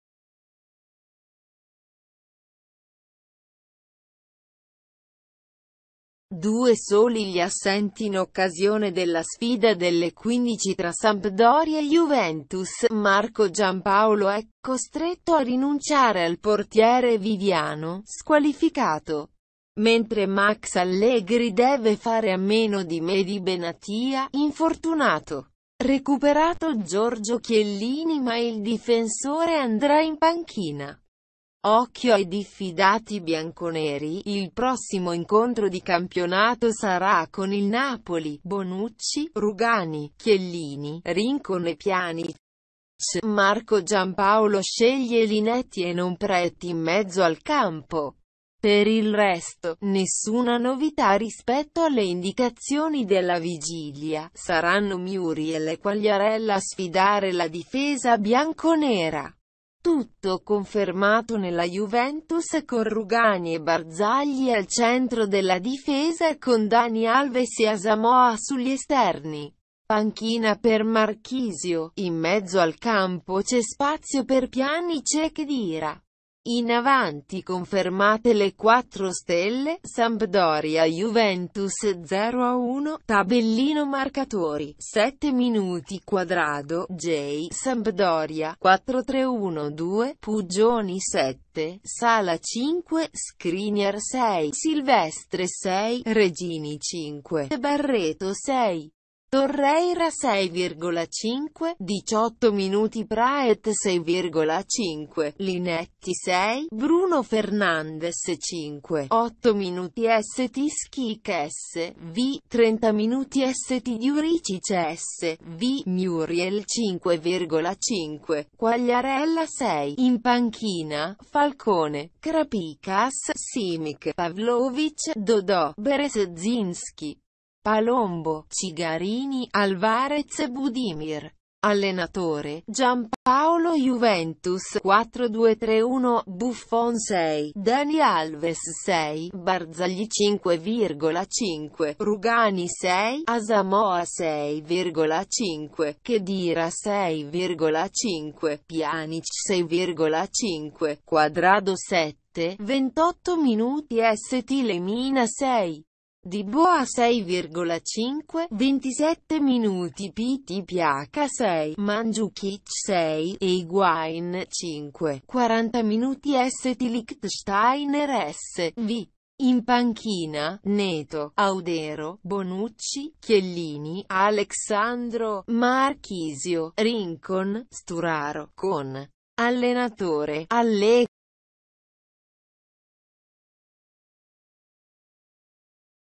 Sampdoria – Juventus 0-1: serie A 19/03/2017 Report
Due soli gli assenti in occasione della sfida delle 15 tra Sampdoria e Juventus: Marco Giampaolo è costretto a rinunciare al portiere Viviano, squalificato, mentre Max Allegri deve fare a meno di Medhi Benatia, infortunato. Recuperato Giorgio Chiellini ma il difensore andrà in panchina. Occhio ai diffidati bianconeri (il prossimo incontro di campionato sarà con il Napoli): Bonucci, Rugani, Chiellini, Rincon e Pjanic. Marco Giampaolo sceglie Linetty e non Praet in mezzo al campo, per il resto, nessuna novità rispetto alle indicazioni della vigilia: saranno Muriel e Quagliarella a sfidare la difesa bianconera. Tutto confermato nella Juventus con Rugani e Barzagli al centro della difesa e con Dani Alves e Asamoah sugli esterni. Panchina per Marchisio, in mezzo al campo c’è spazio per Pjanic e Khedira. In avanti confermate le 4 stelle.